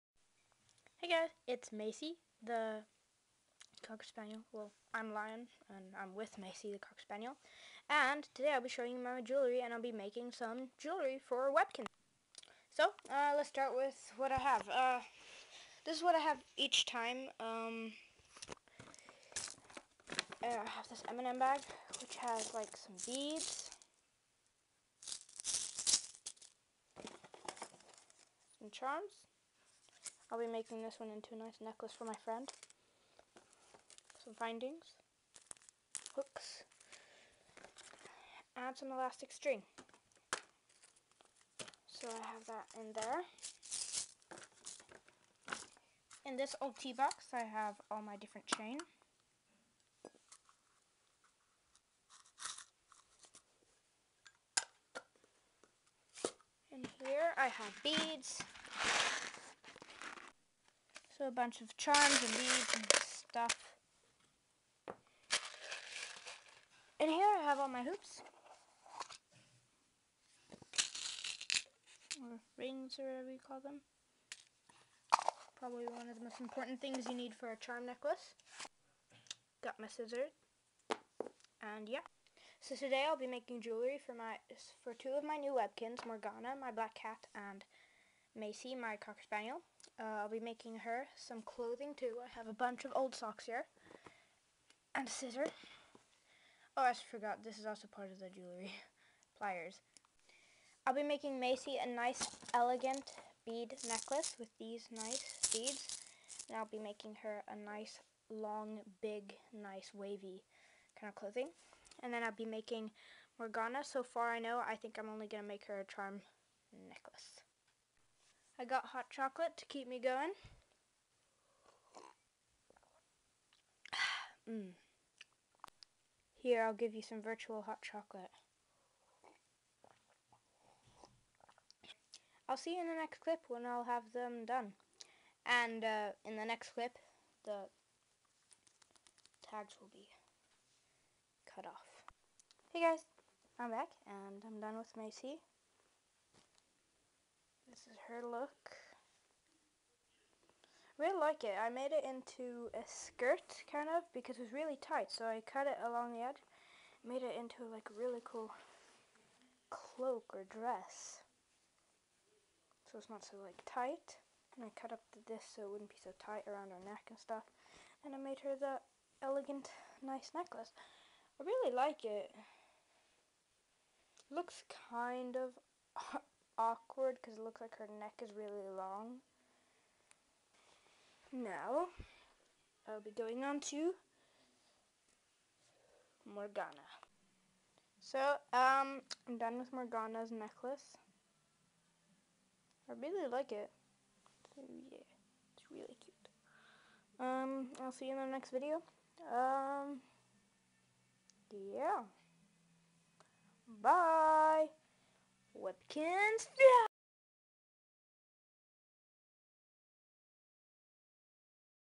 Fashion for DA new Kinz!
In this video I just make some little nice things for my wonderful Webkinz!